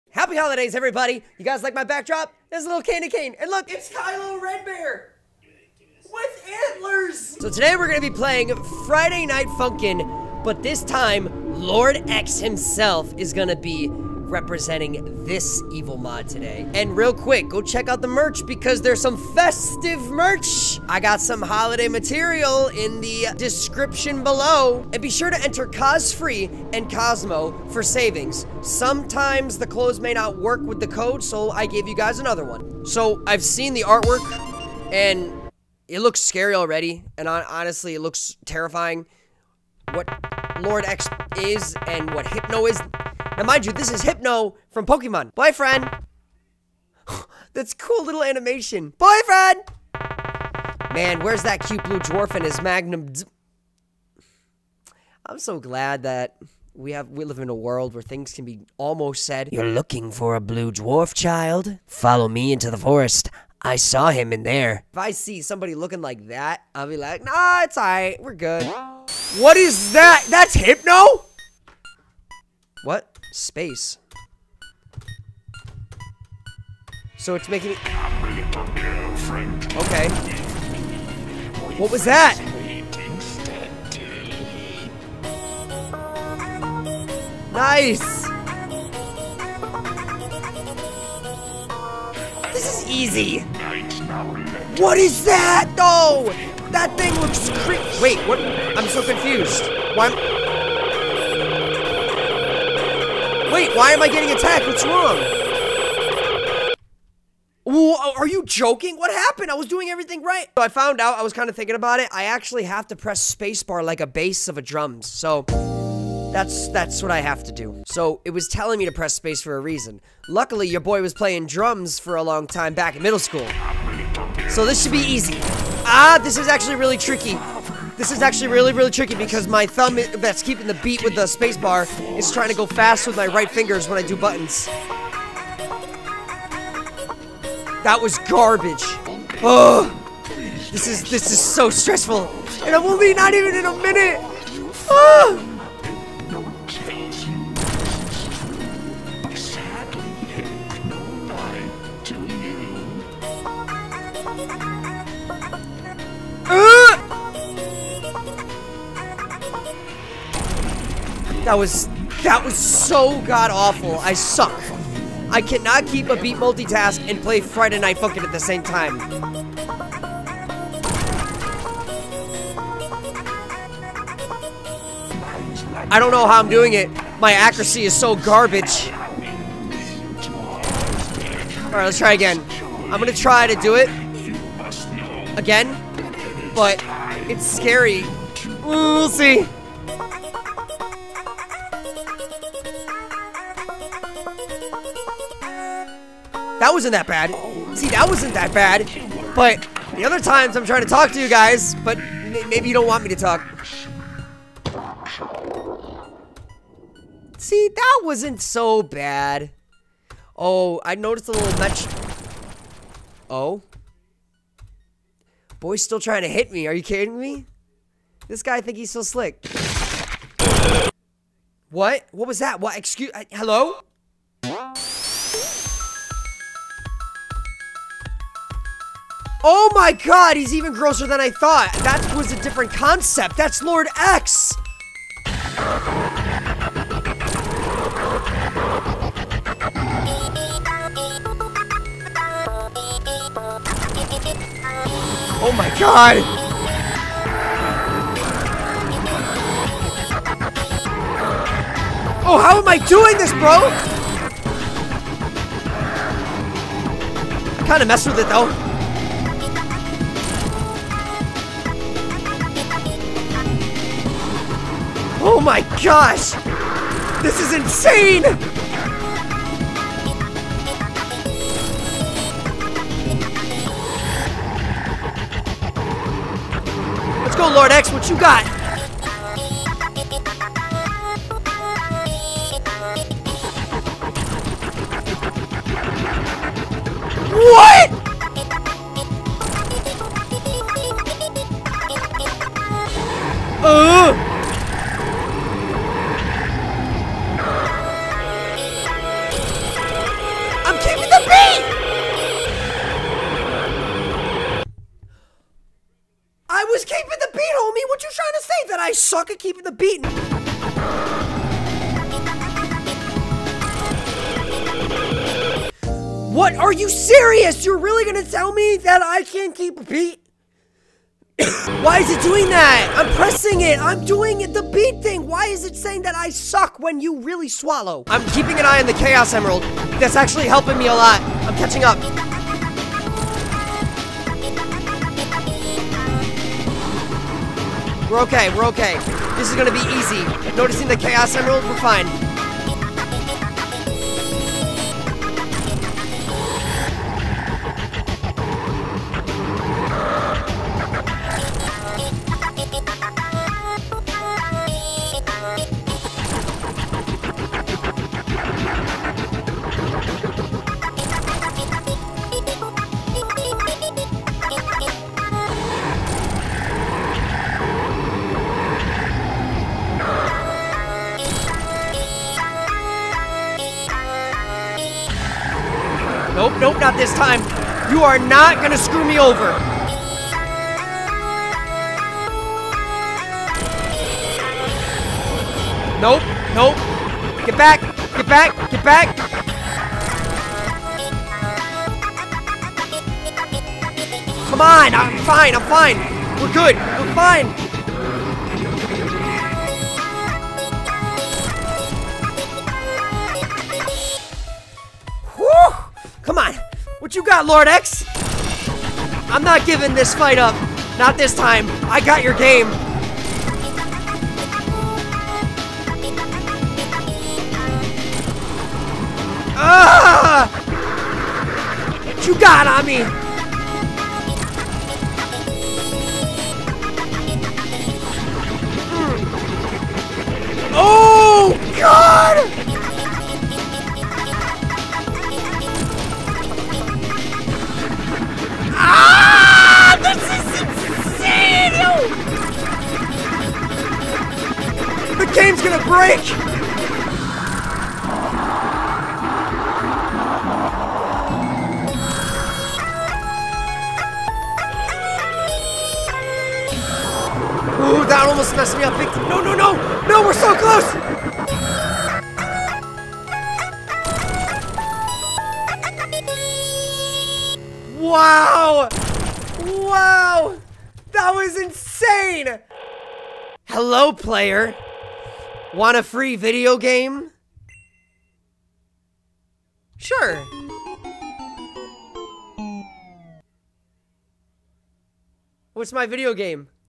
Lord X makes an appearence!! *THIS BEAT IS FIRE*
I was completely thrown off! Lord X is here! this is wild! but it seems that Hypno has something to do with this!! LETS SLAY THIS TRACK